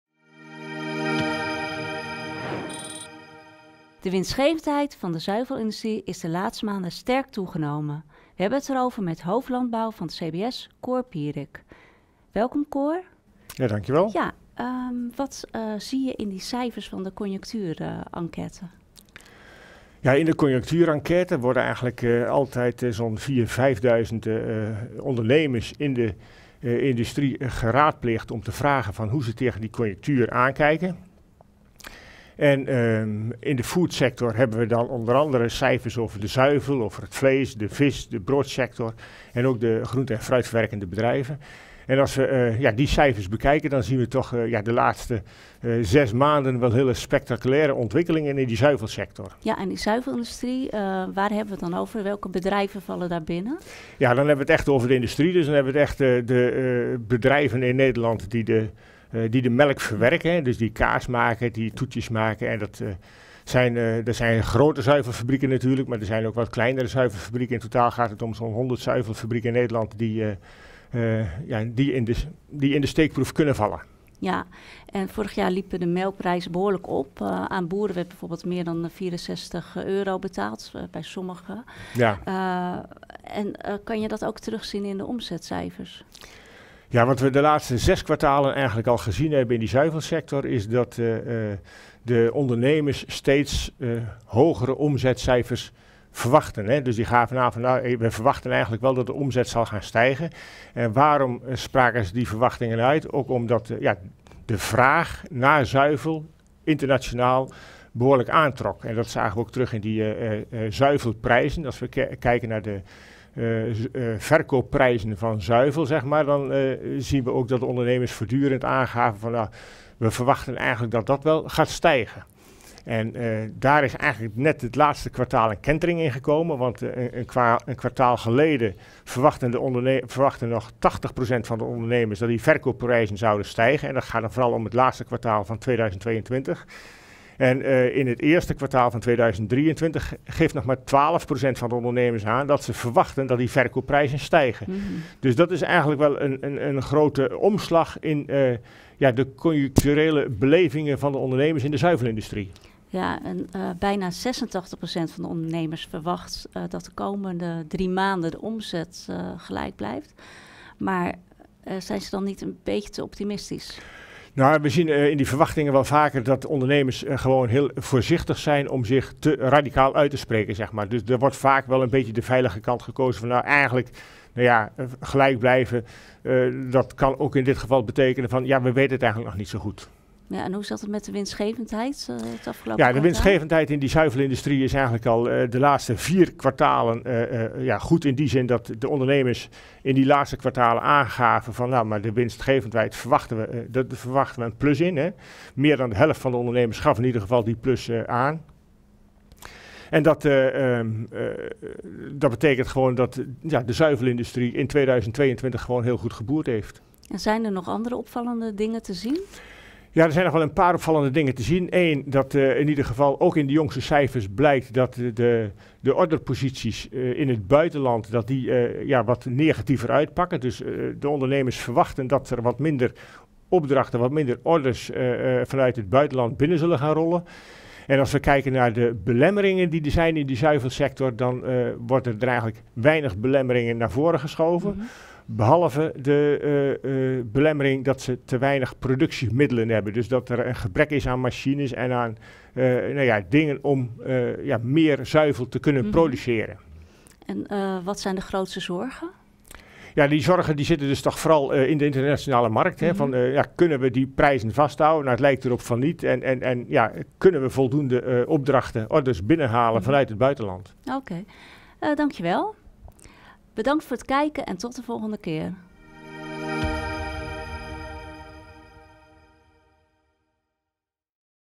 BB TV - BB Feiten met Cor Pierik - 'Omslag in verwachting over verkoopprijzen zuivel'
Zuivelfabrieken verwachtten het laatste anderhalve jaar steeds hogere omzetcijfers. "De vraag naar zuivel trok internationaal behoorlijk aan en dat zagen we terug in de zuivelpijzen", licht Cor Pierik, hoofd Landbouw van het CBS, toe. In het eerste kwartaal van 2023 geeft nog maar 12% van de ondernemers in de zuivelindustrie aan dat ze verwachten dat de verkoopprijzen stijgen.